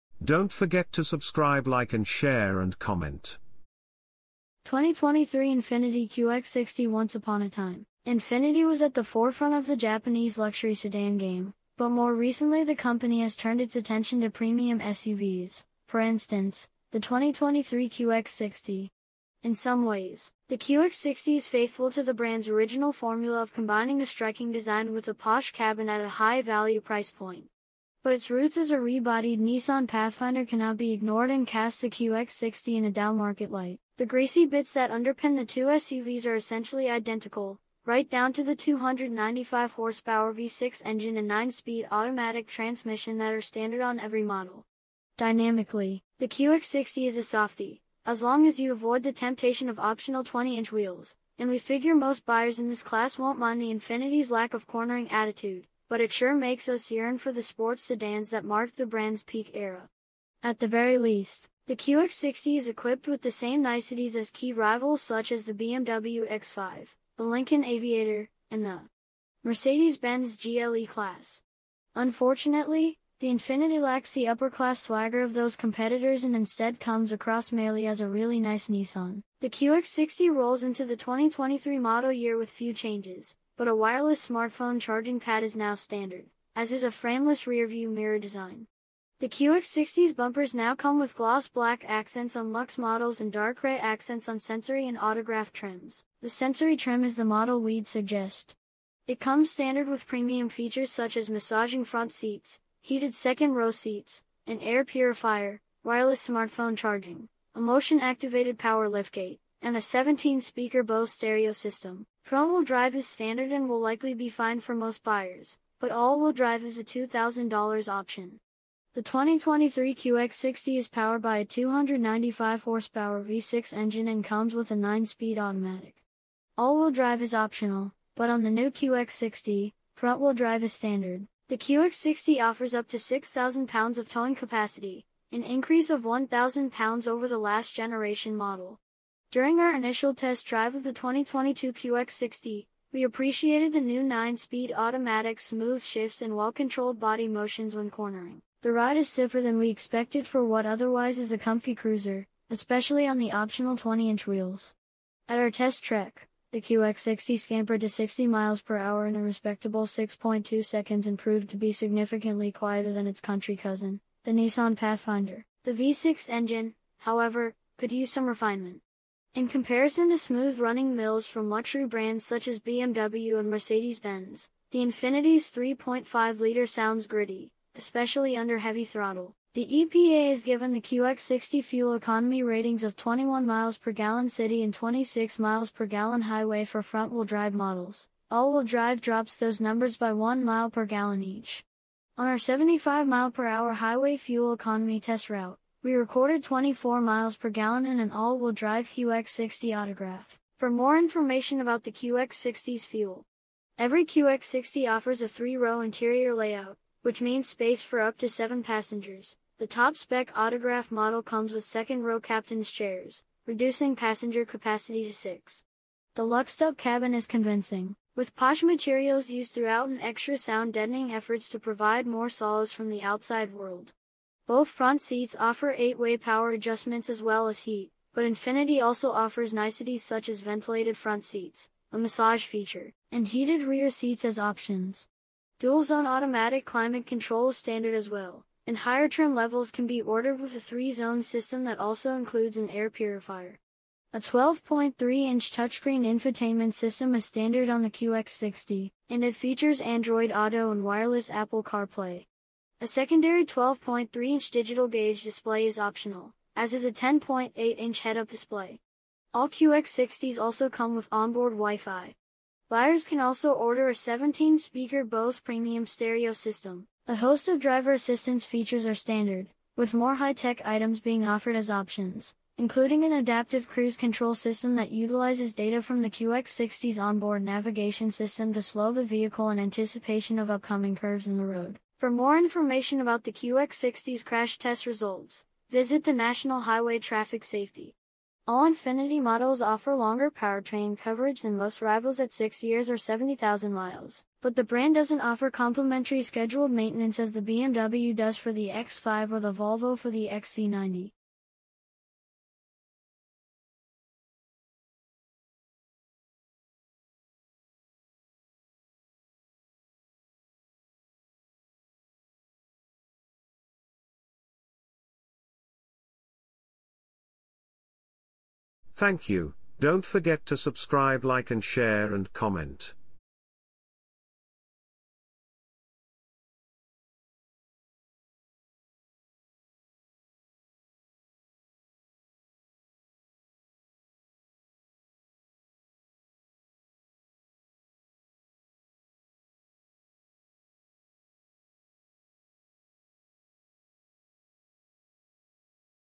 Is Infiniti the Best? Using 3-Line Luxury Infiniti QX60 Review for 2023
The QX60 is a luxury Crossover SUV but is it better than a BMW? Learn about the 2023
Meet the 2023 INFINITI QX60 Sensory! This new luxury 3-row was just redesigned last year and for its second model year, it received a couple refinements. So, does this beat the Acura MDX and Lexus RX??
Considering buying a 2023 Infiniti QX60? This sensory trim is the model I would suggest. Loaded with features like massaging front seats, air purification, a wireless Qi charging system and a upgrading Bose audio system! Is it still trying to stand taller than the Nissan Pathfinder, it offers an easy to follow interior layout and loads of passenger volume. That wow factor is certainly there but does it offer enough panache to compete in the luxury SUV market!
Infiniti best selling vehicle of any description is the QX60 which was recently redesigned under Infinit's new "Nissan Plus" design philosophy. I find the talk of Nissan Plus refreshing actually because let's be honest, the Lexus RX is "Toyota Plus" and the Acura MDX is certainly "Honda Plus" even though those companies won't talk in those terms. Infinit's new mantra is seen in the pricing structure of the QX60 and its practical interior features which are largely shared with the Pathfinder, but does that make this the CUV to beat? If you have kids, its an easy yes.